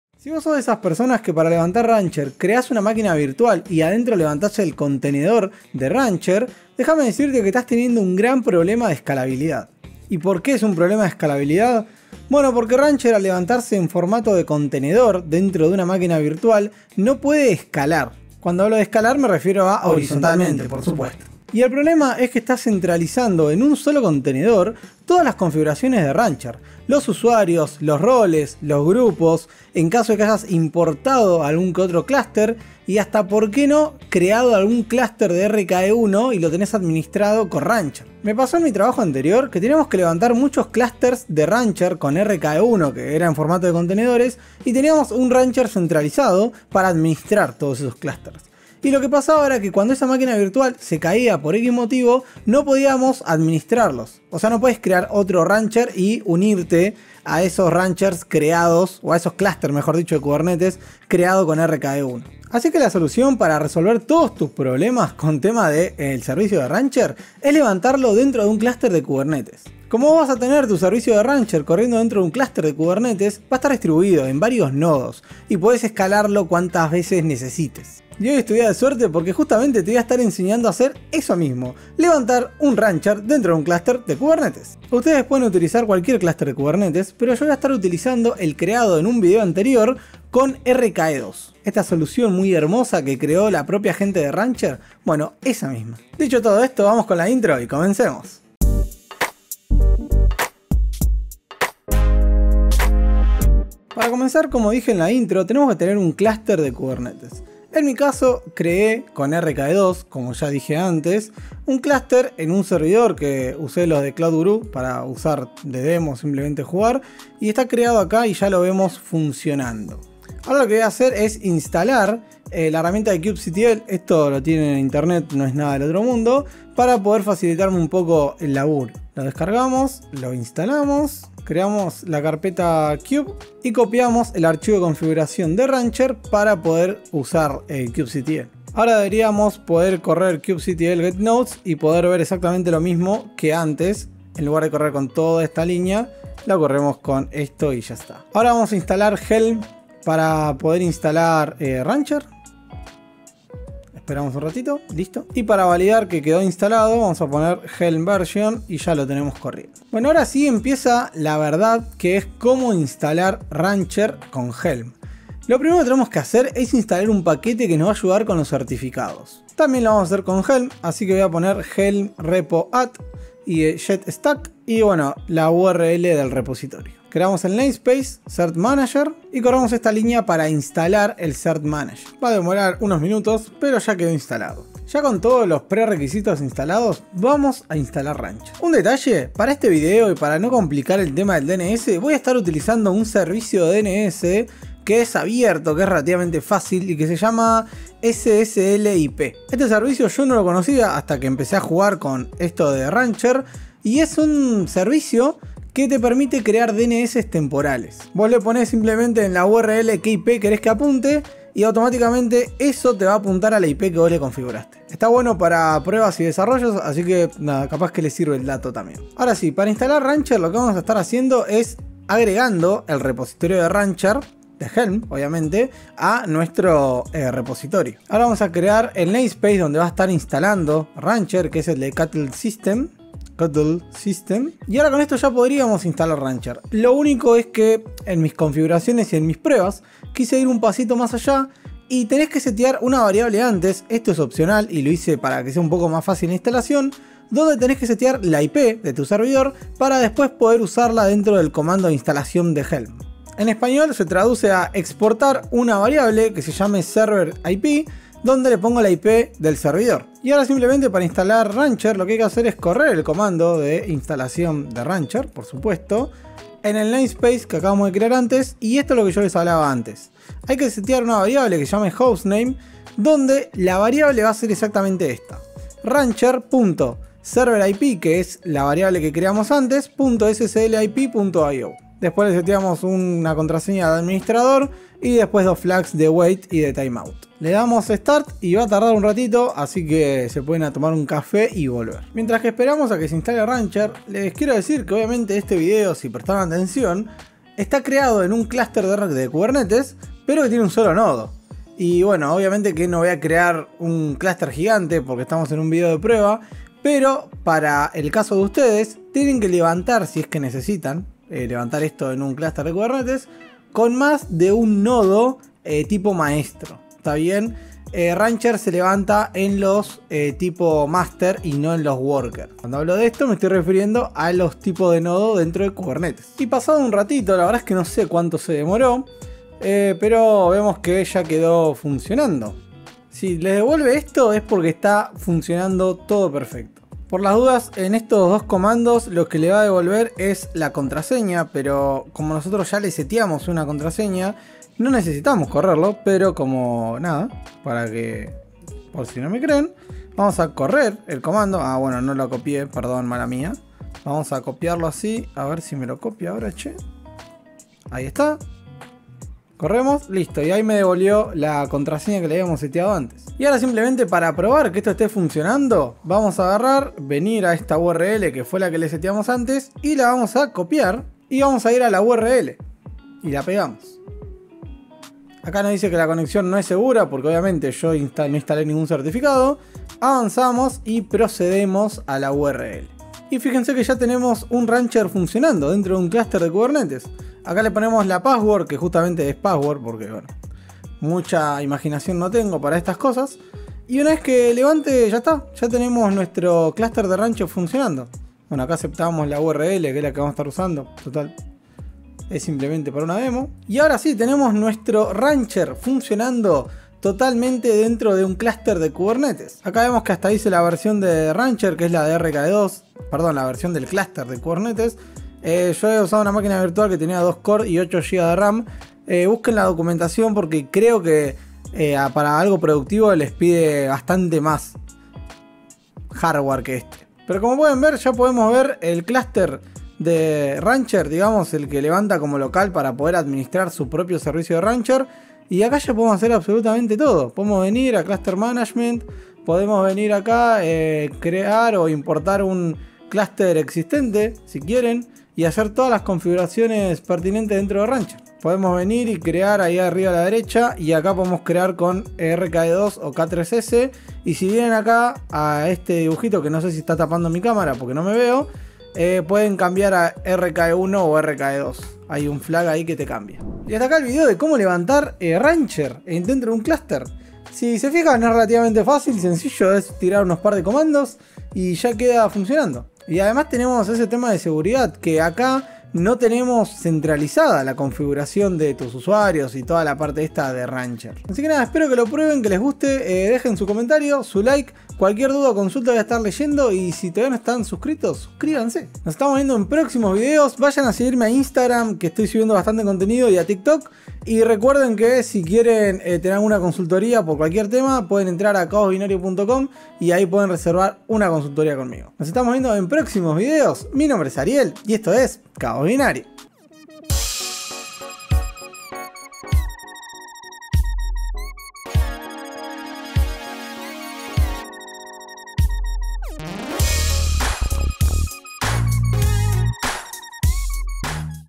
RANCHER CORRIENDO EN KUBERNETES
Hoy vamos a estar instalando Rancher en un clúster de Kubernetes de RKE2.